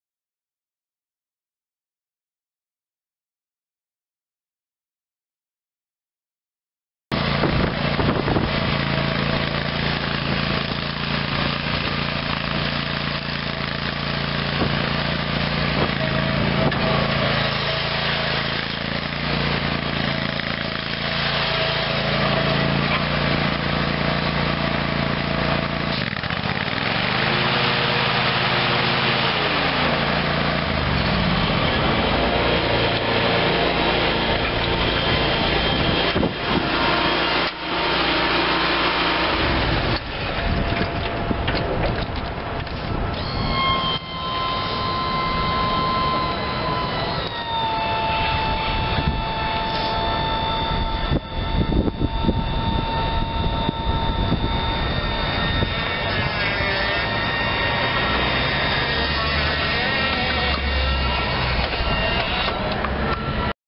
Hustler Flip-Up ZTR lawn mower for sale at auction | bidding closes May 16, 2018
Hustler Flip-Up ZTR lawn mower
1 hours on meter
48" cut
Kawasaki 25 HP gas engine
9x3.5x2.25 front tires
20x10x10 rear tires
Possible service issues
Serial 16082252